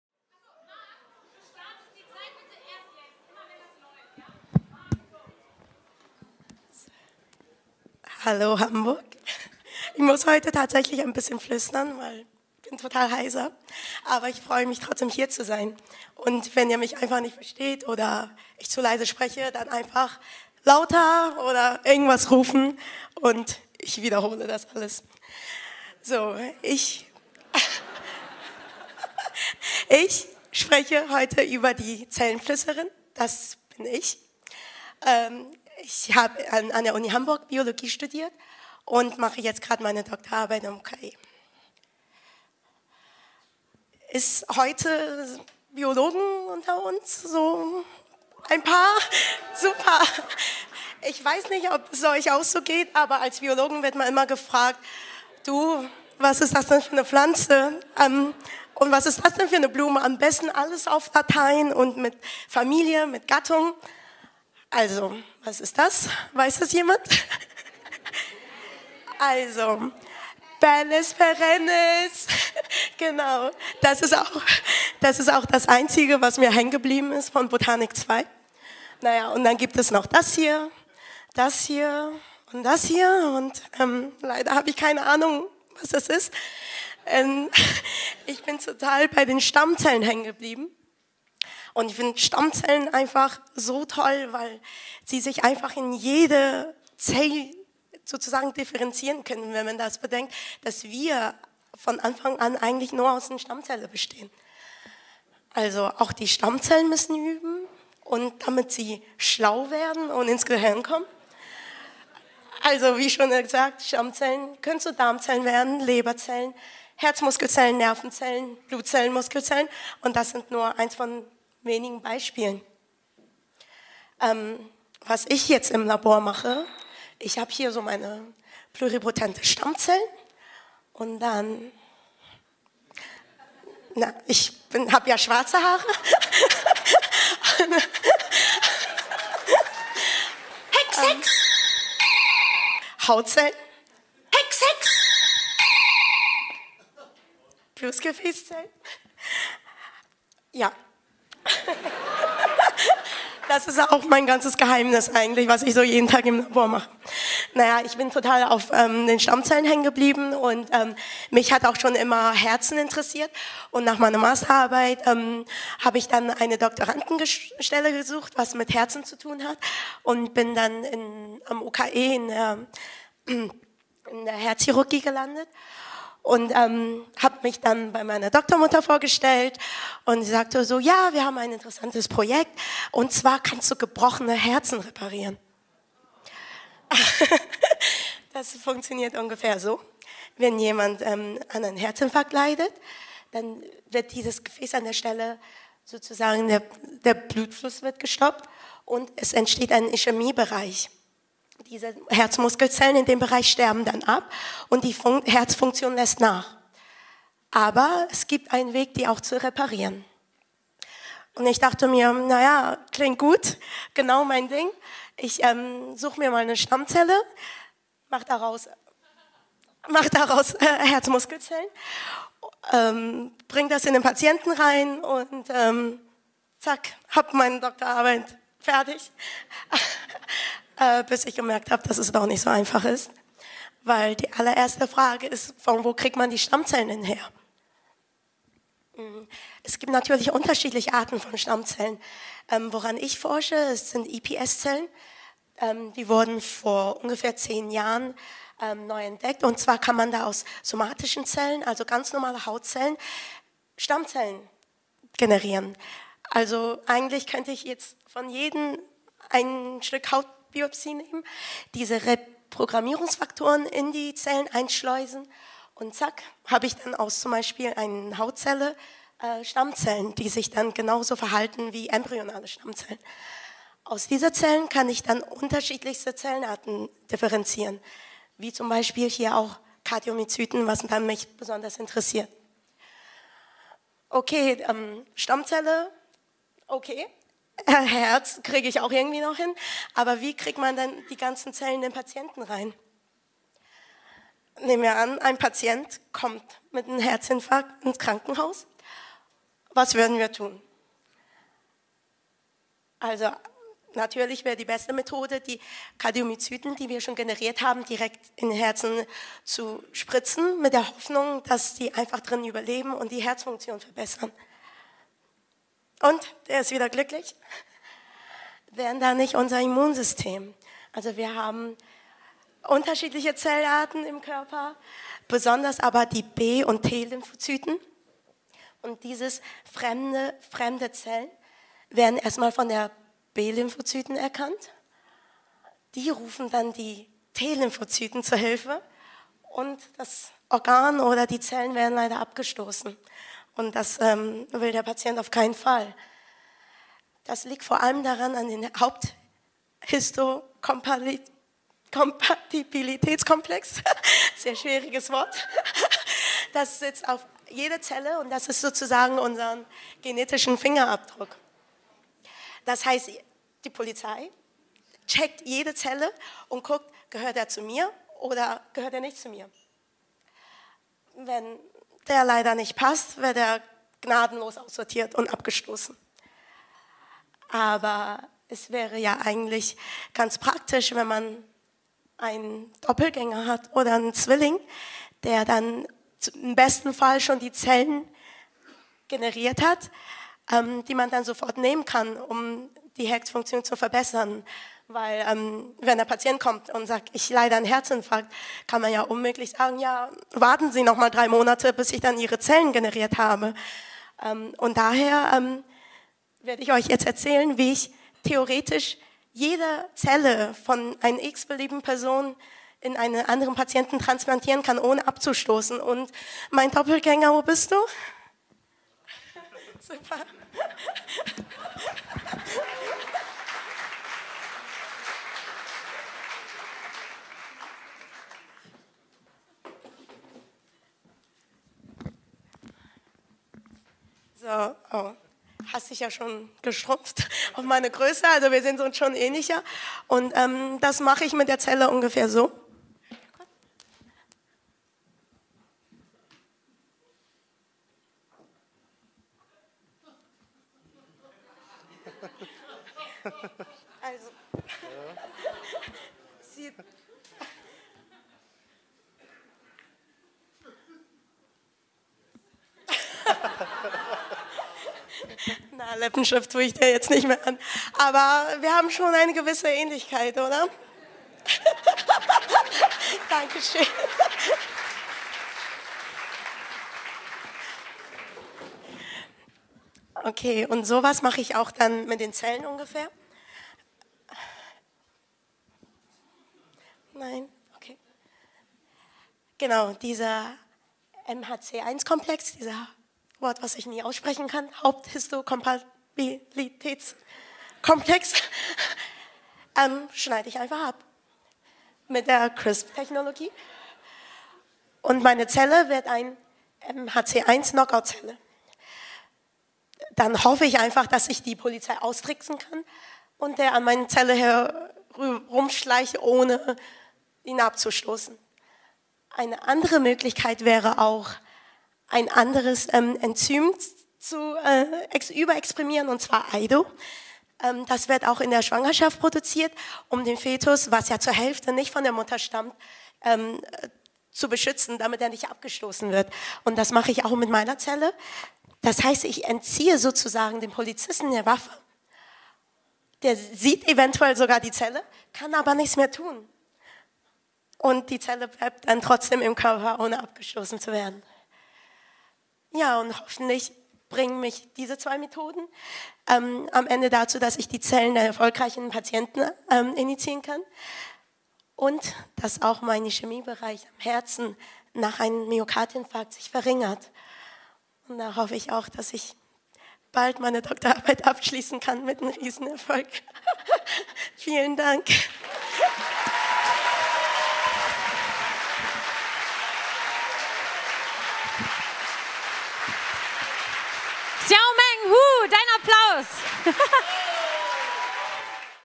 Die Zellenflüsterin (Xiaomeng Hu- Science Slam)
Xiaomeng Hu hat Biologie studiert und erforscht in ihrer Doktorarbeit an Stammzellen. Ihre Untersuchungsfrage; Wie kann man aus Stammzellen Herzmuskelzellen generieren?